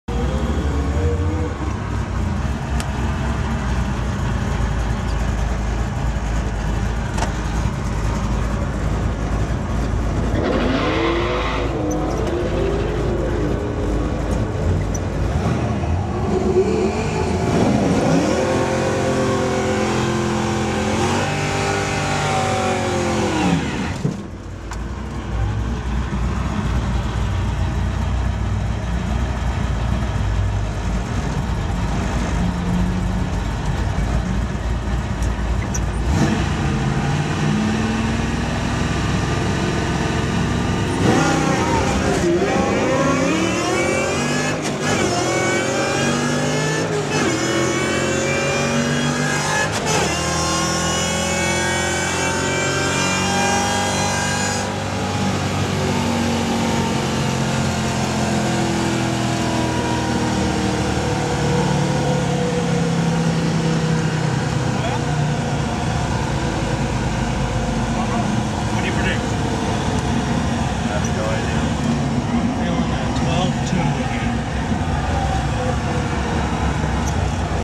2 Camaros Drag Racing at LVMS
a '67 Camaro racing a '72 Camaro at Las Vegas Motor Speedway!  These 2 bad boys are totally street legal but are always grudging it out at the drag strip!